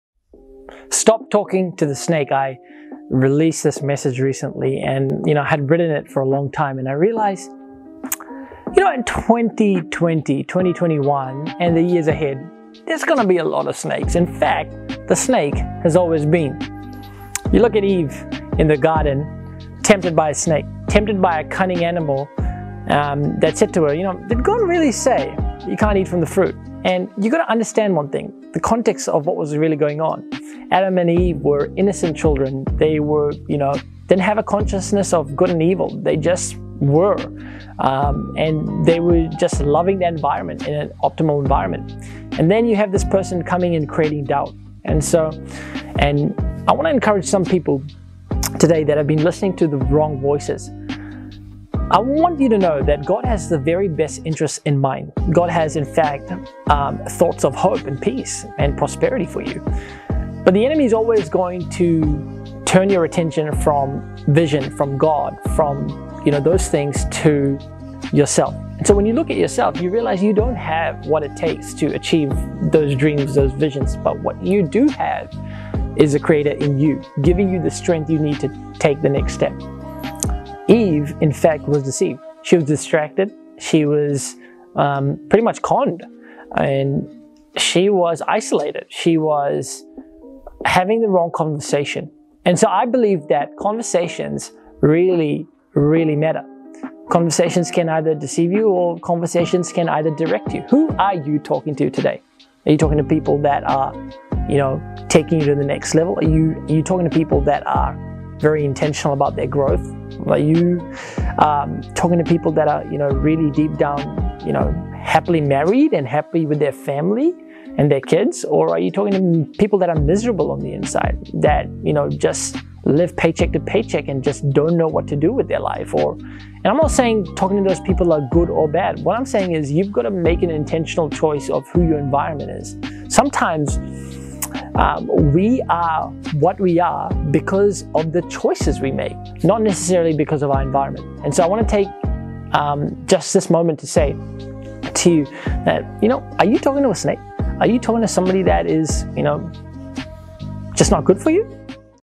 Stop Talking to Snakes - Wisdom Wednesday - Ep 109
Are you having conversations with snakes? With people that create doubt, with people that are bitter? With people that constantly gossip? Who you choose to dialogue with is key to a healthy life.

Conversations matter

It consists of 31 secrets to living a more fulfilled life.
You never know what works until you try!